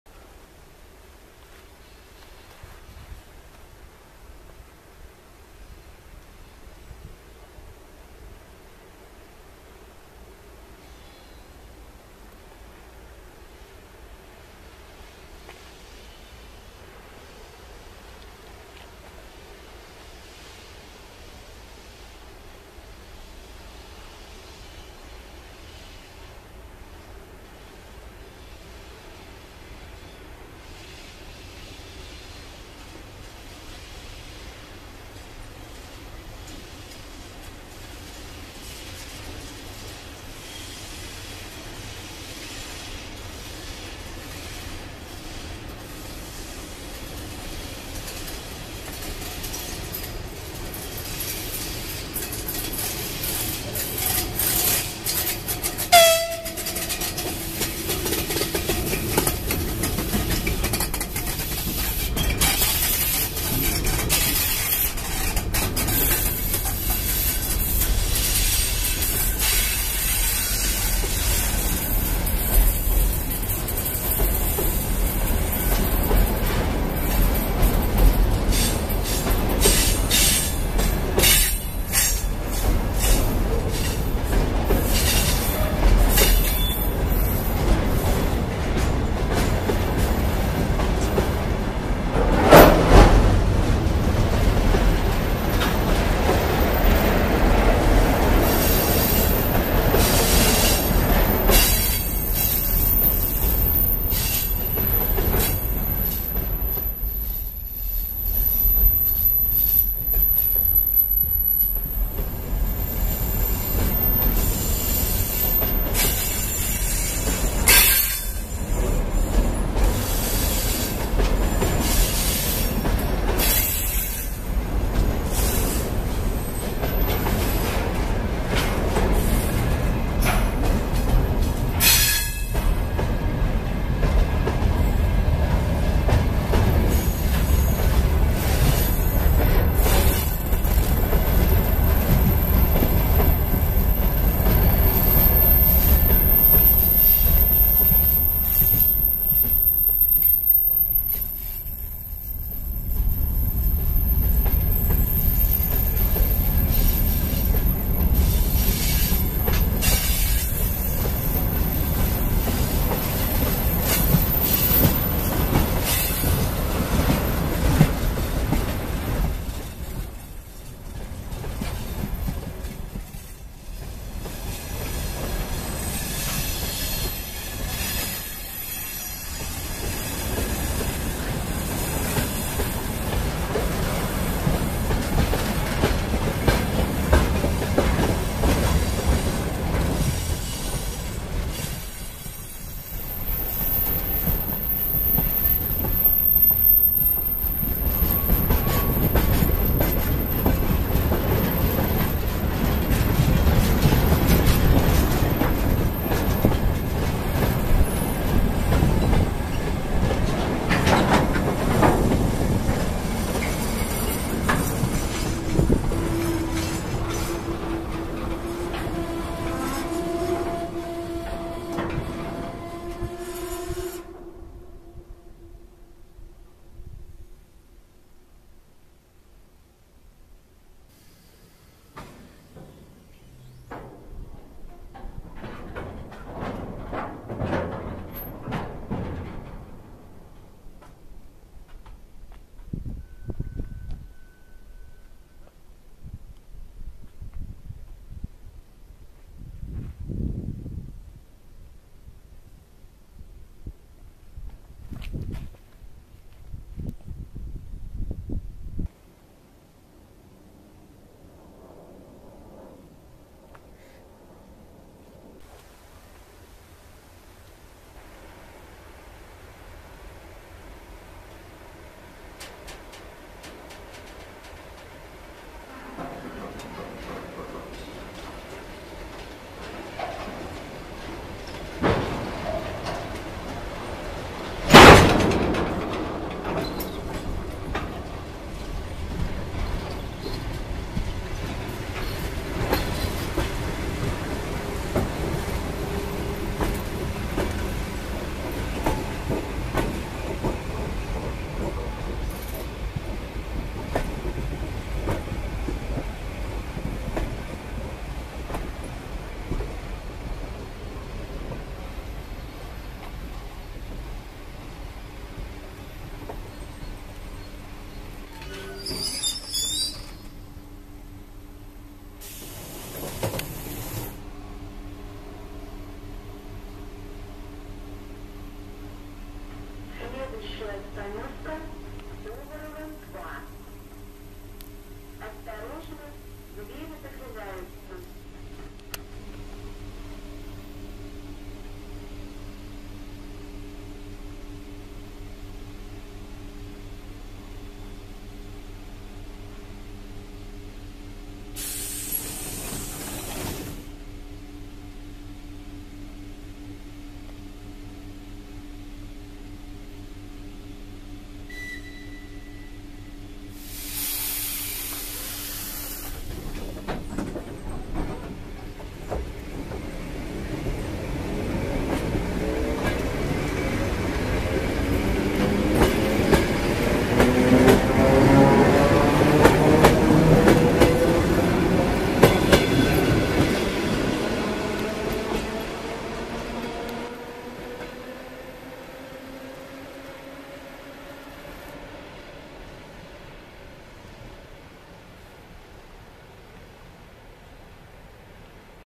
Электровоз ВЛ10У-747 с грузовым поездом и электропоезд ЭР2Р-7023
ВЛ10У-747 - утяжелённый электровоз, колёса которого имеют большую силу сцепления с рельсами, благодаря чему он способен возить более тяжёлые составы с грузовым поездом и ЭР2Р-7023 - пригородный рижский электропоезд постоянного тока.

Снято на платформе 142 км.
Большая Московская Окружная Железная Дорога. Московская область.